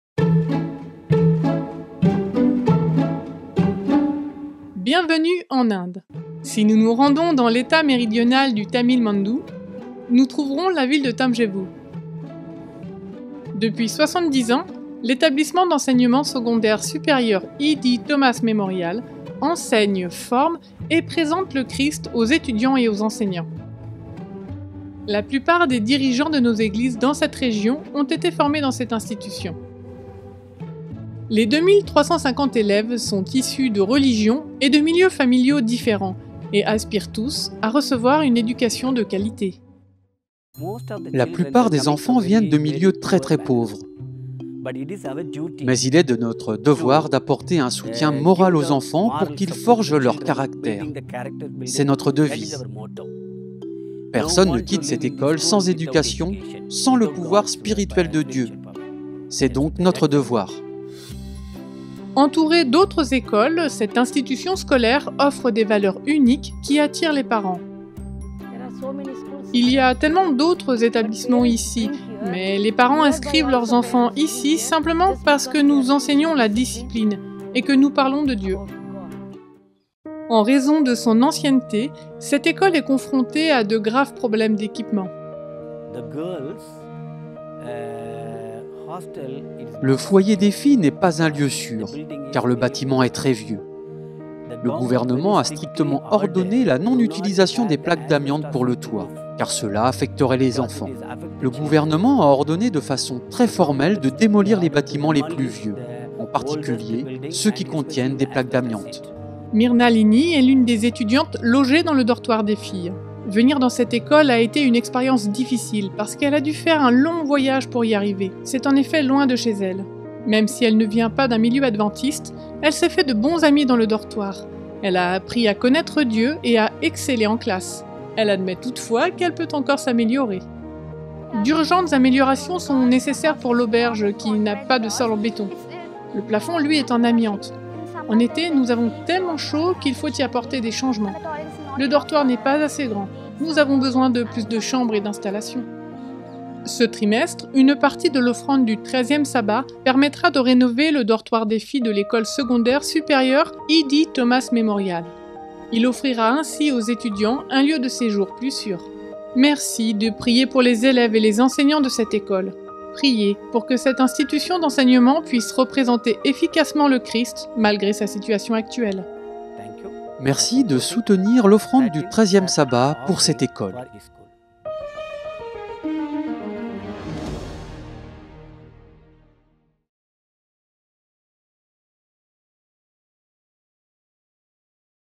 La démolition du dortoir
En raison de la vétusté de ses installations, en particulier du dortoir des filles qui contient des plaques d'amiante, l'école secondaire supérieure E. D. Thomas Memorial doit relever certains défis. Une partie de l'offrande du 13e sabbat sera utilisée pour rénover le dortoir afin de créer un environnement plus sûr et plus confortable pour les étudiantes.